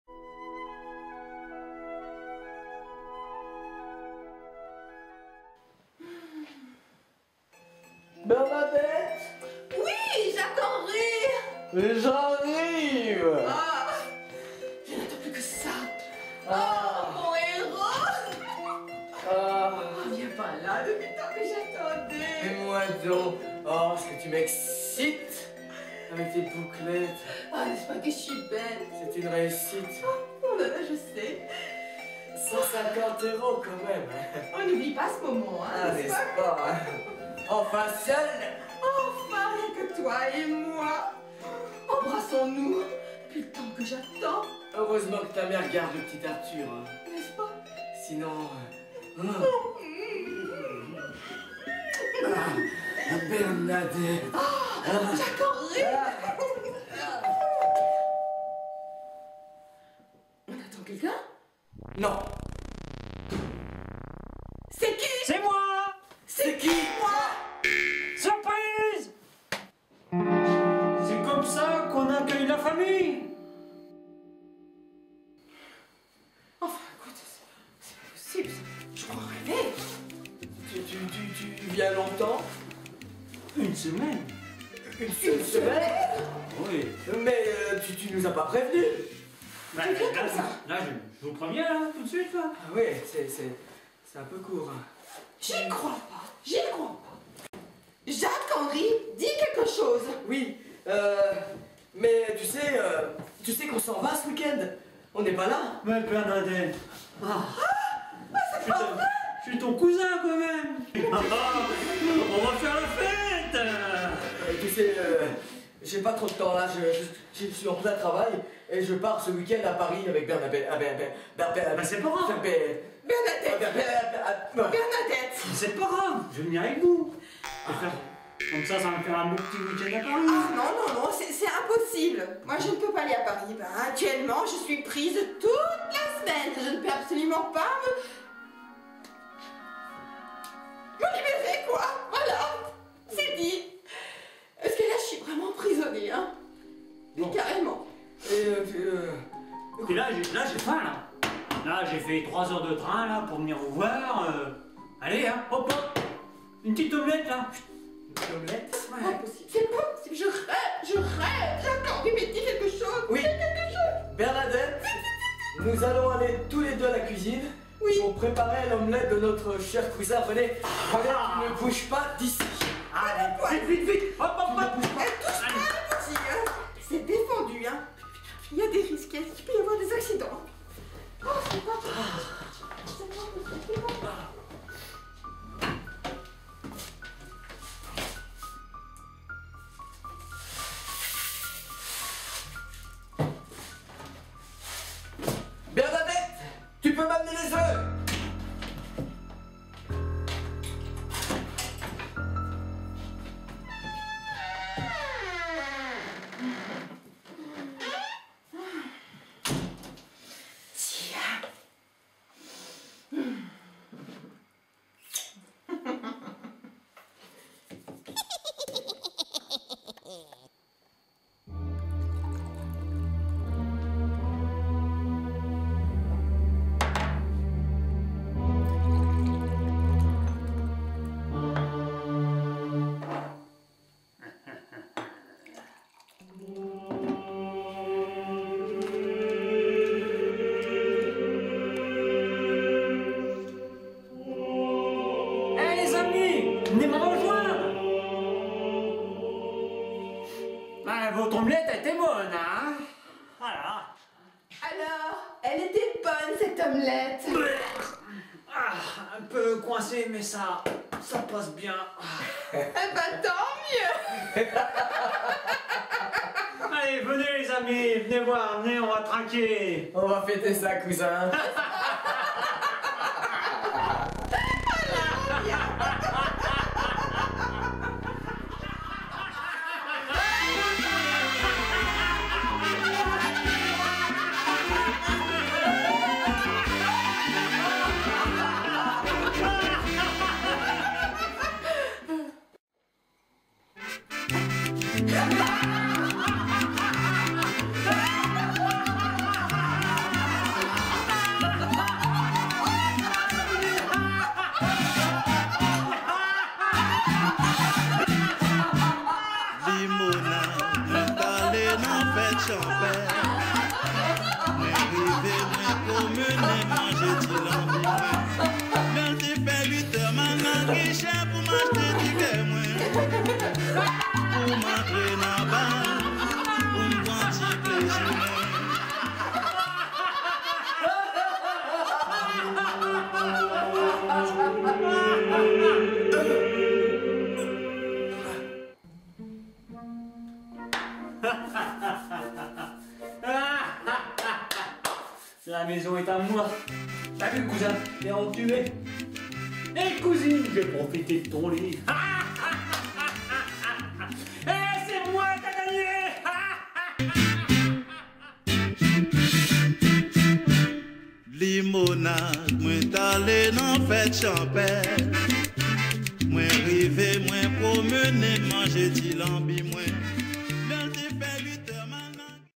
Soirée coquine ;)))
Alors que Bernadette et Jacques Henri pensaient être seul pour une soirée érotique, voilà que débarque chez eux un lointain cousin, emmerdeur et sans gêne...  ;))))